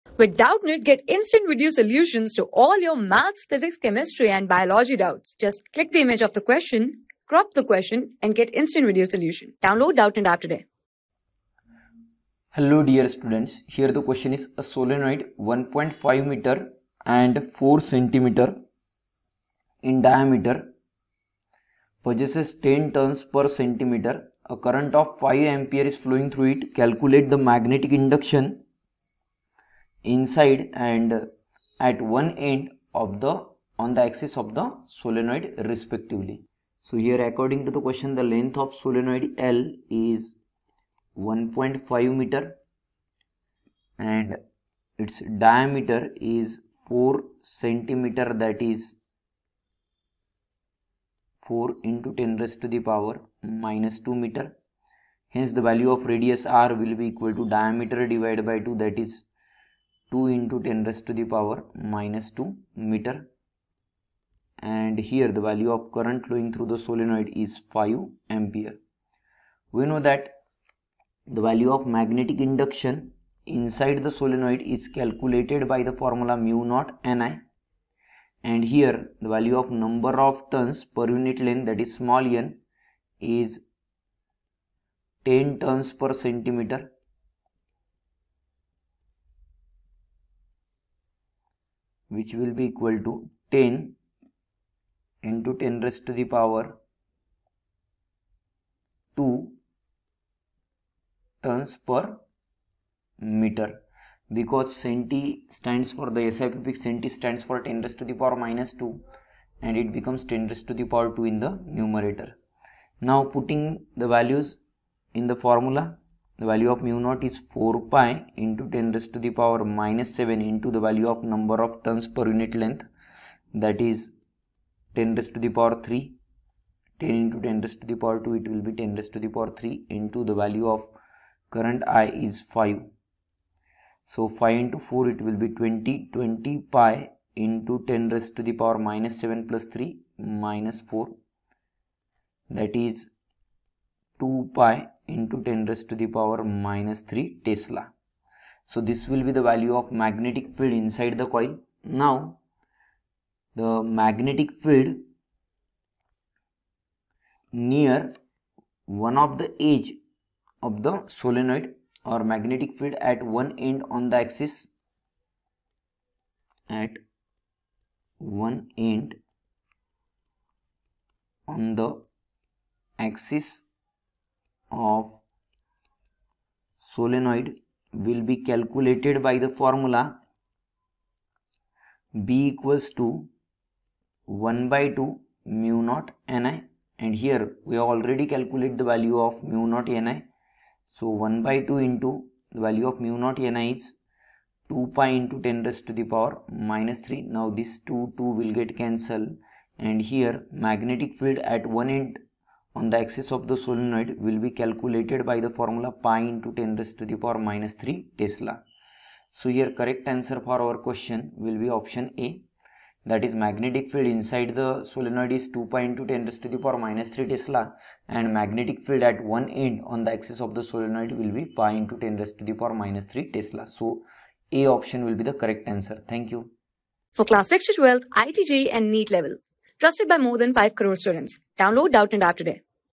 A solenoid 1.5 metre and 4.0 cm in diameter possesses 10 turns/cm. A current of 5.0 A is flowing...
A solenoid 1.5 metre and 4.0 cm in diameter possesses 10 turns/cm. A current of 5.0 A is flowing throught it. Calculate the magnetic induction (i) inside are (ii) At one end on the axis of solenoid respectively
Class: 12
Subject: PHYSICS
Chapter: MOVING CHARGES AND MAGNETISM
Board:IIT JEE

👉 Have Any Query? Ask Us.